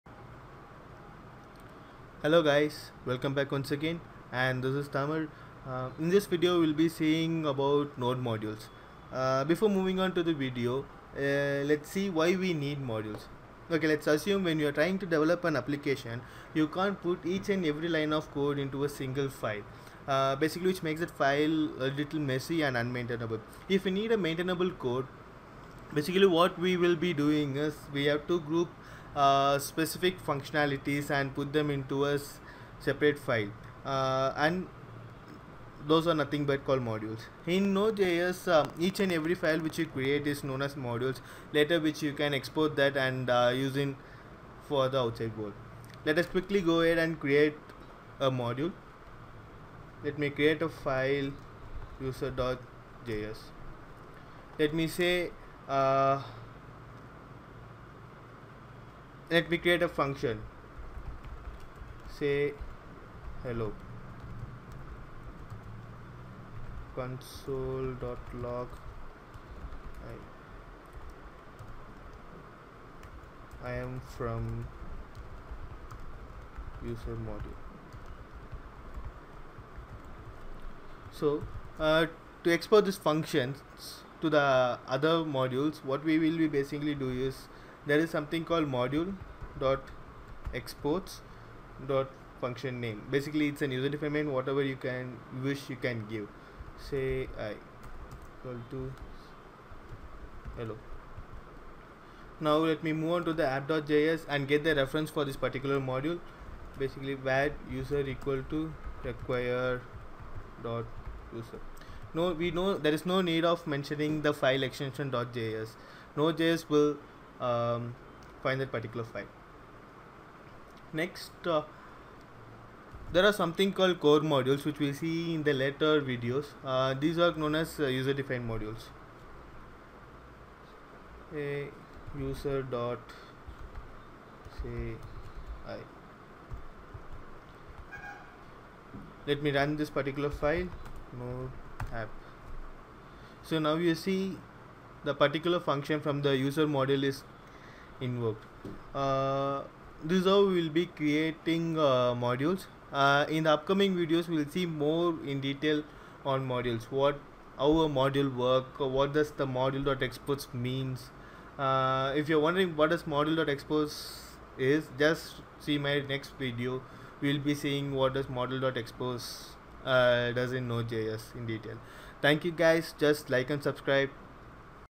Node.js Tutorial #5 - Creating modules
This video explains how to create modules in node.js